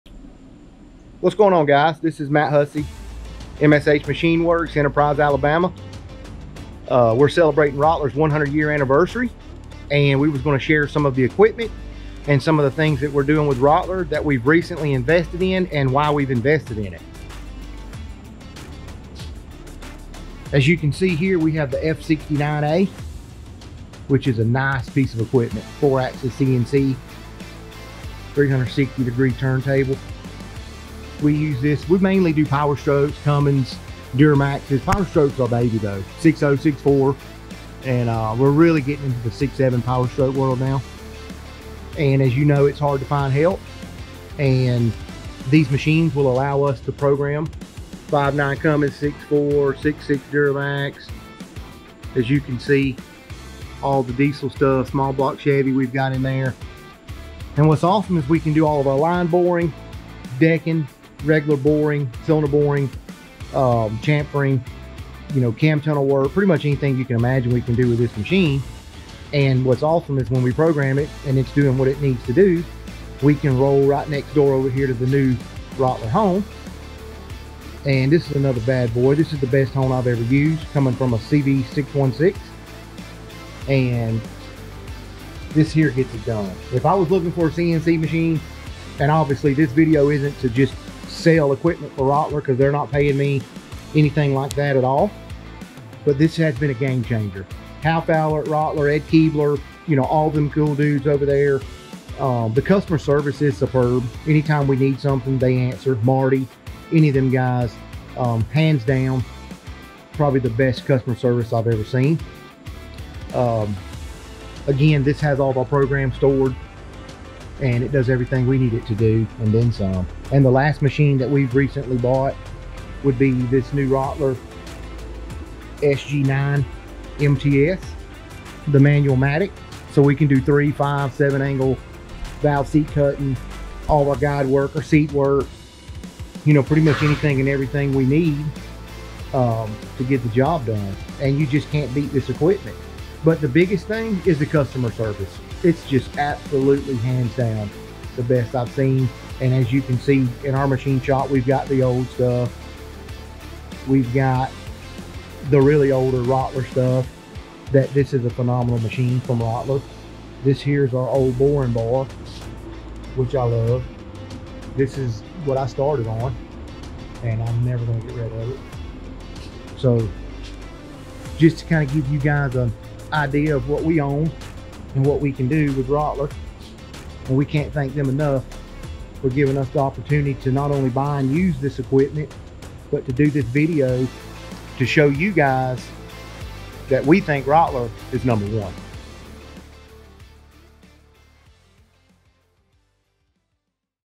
Rottler Report with Matt Hussey with MSH Machine Works
Matt Hussey is the owner of MSH Machine Works, an engine machine shop located in Enterprise, AL.  They recently purchased a new Rottler Manufacturing F69A to help machining the diesel engines they regularly build, from Powerstrokes to Duramax and Cummins.

"We think Rottler is #1. The dudes over there are amazing and the customer service is superb."

Hear more from Matt about the shop and his new purchase in this Rottler Report!